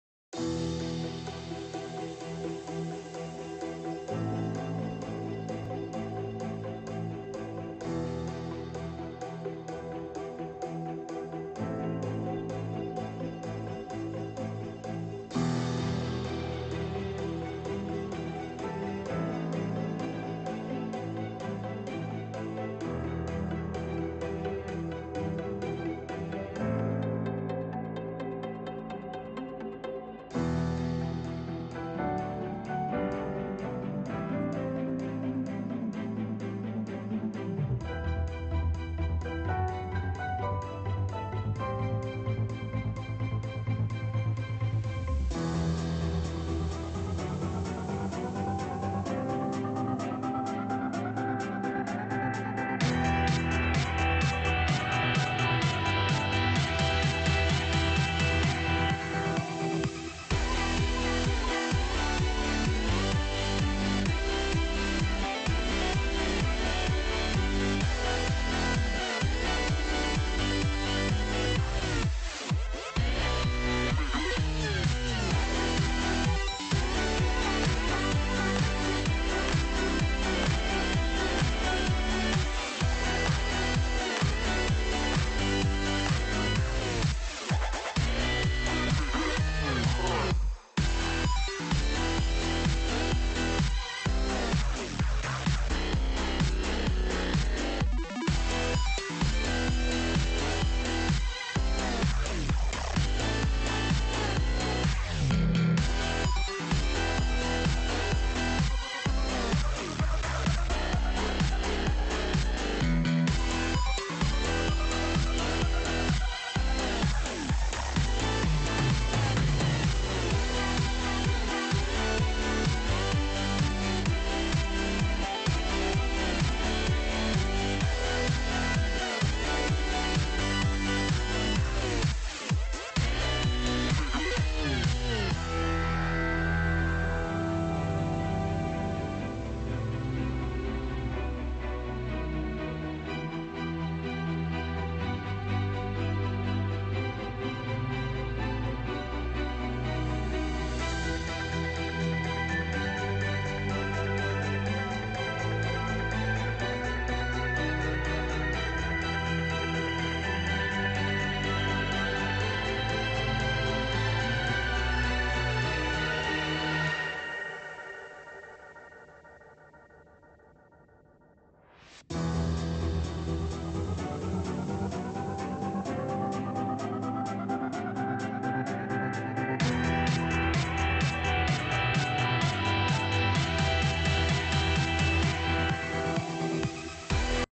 When you accidentally open a legendary chest....
I cant believe this happened! I was saving the chest for an opening but i guess not...